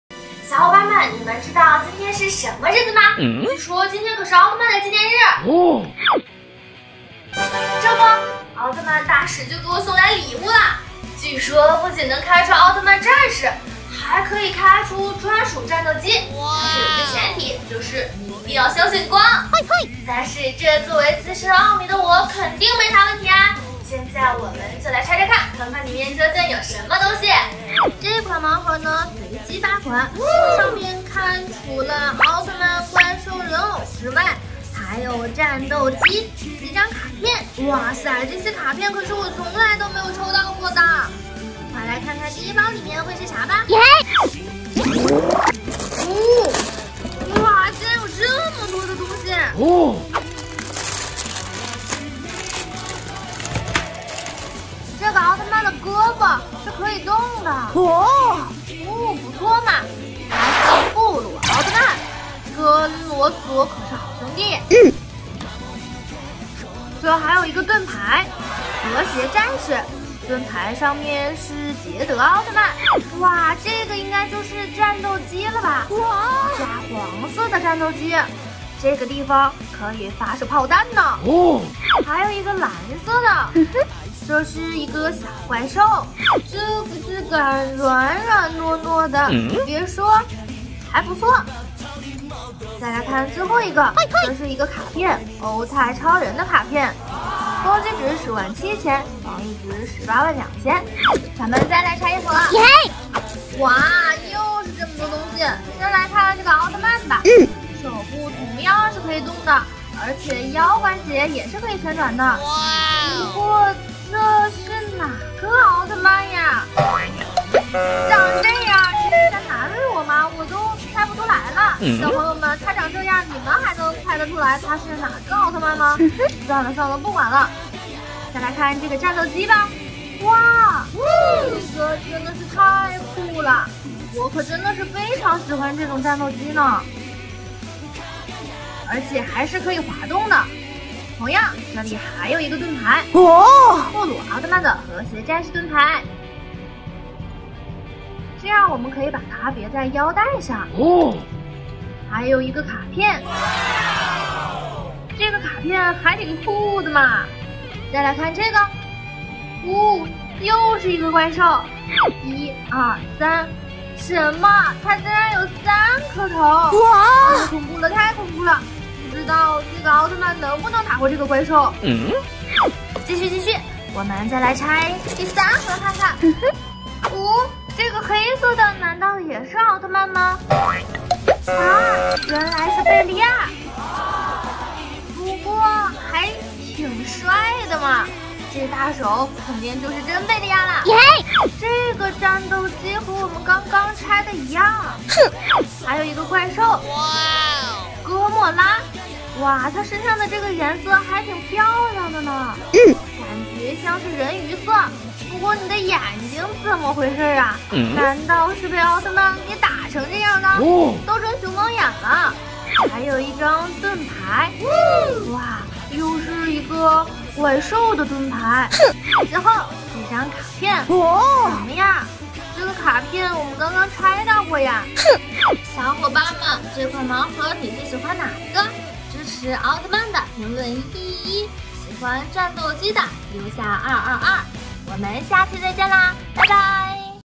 开箱奥特曼百变超人盲盒，居然开出贝利亚和战斗机，这是奥特曼吗
玩具开箱又来啦，本期是奥特曼百变超人系列盲盒，会开出什么好东西呢？